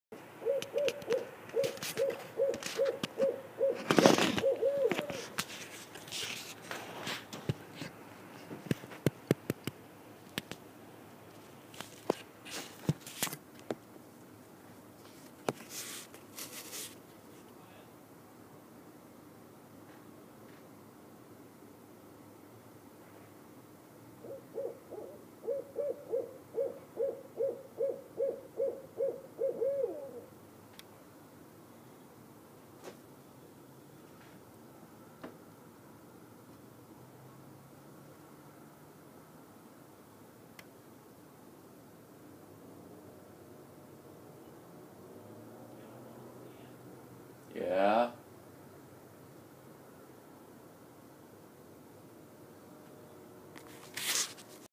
Owl outside my window
Random owl that always shows up outside my window